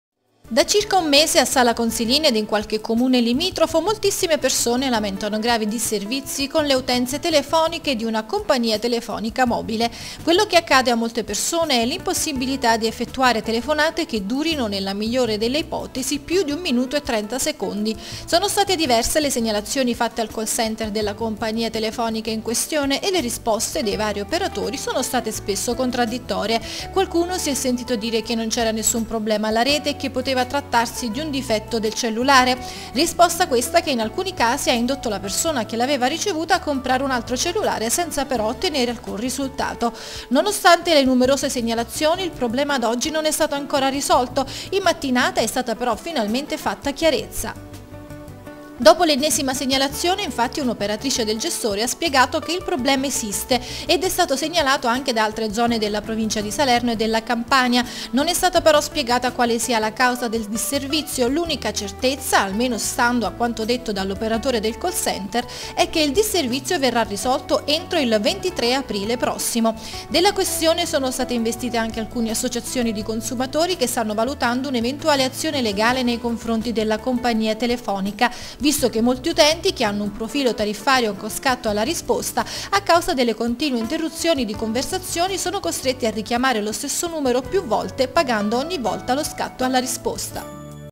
Italia 2 Tv 26/3/2010 Sala Consilina. Disservizi causati da un gestore di telefonia mobile..mp4
Sala Consilina (SA): insorgono gli utenti a causa dei disservizi causati da un gestore di telefonia mobile.